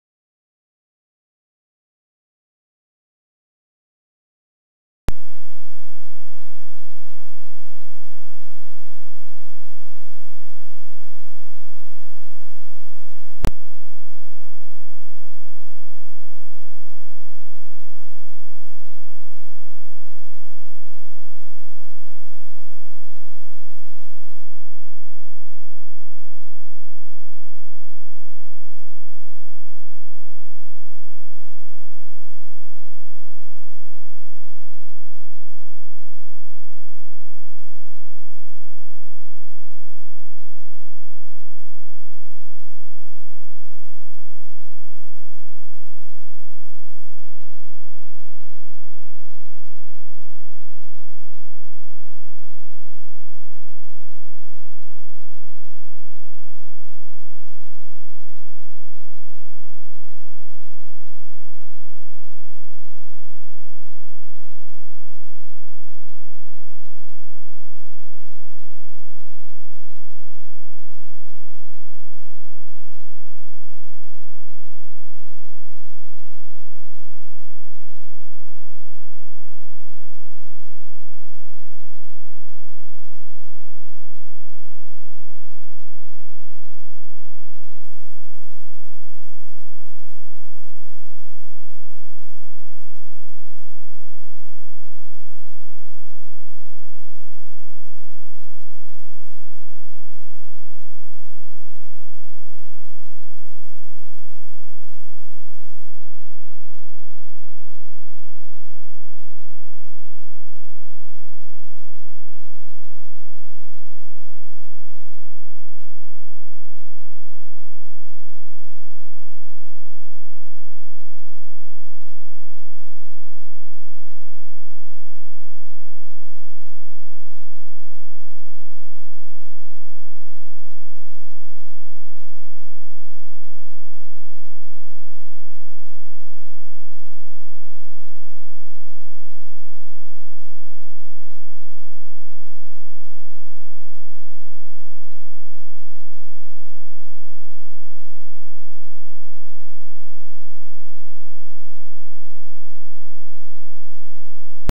Phono Oscillator, Steampunk style, combined with a Mendocino Motor
Building a phono oscillator with tubes, steampunk style, combined with a Mendocino Motor